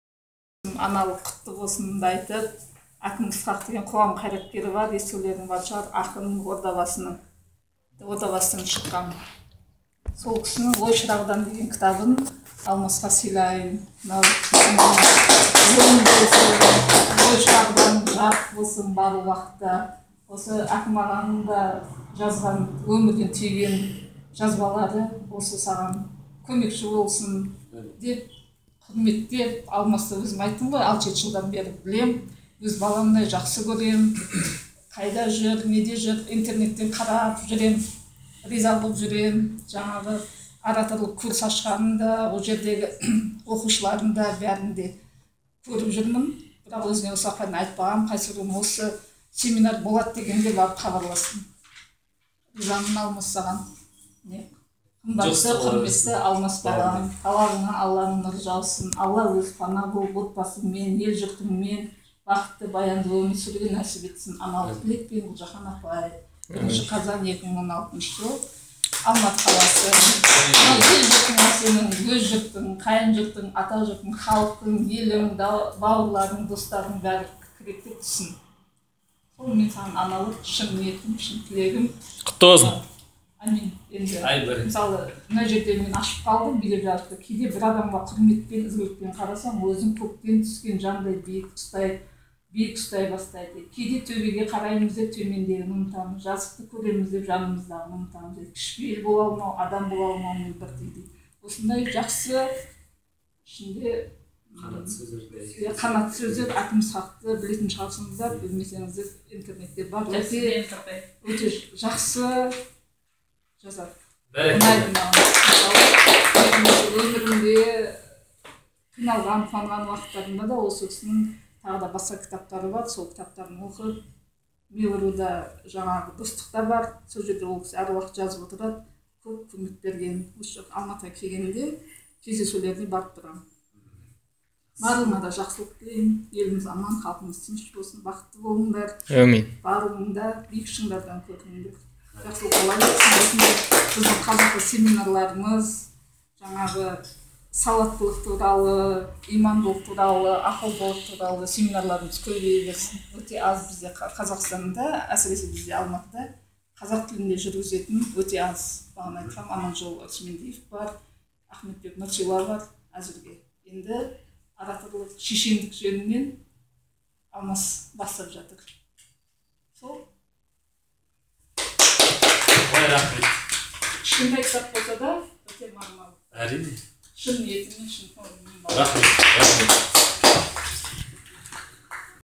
Біздің семинардан шабыт алған Гүлжаһан апайымыздың жылы лебізі
Гүлжаһан апаймен біраз жыл болған еді, кездеспей кеткеніме, бүгін міне біздің семинарға келіп, өзінің тартуын беріп бізді тағы да қуантып қойды. Ол кісіге мың алғыс. Аллаға шүкір осында әпкелеріміздің арамызда бар болғаны үшін. Ол кісіден үйренетініміз әлі ақ көп!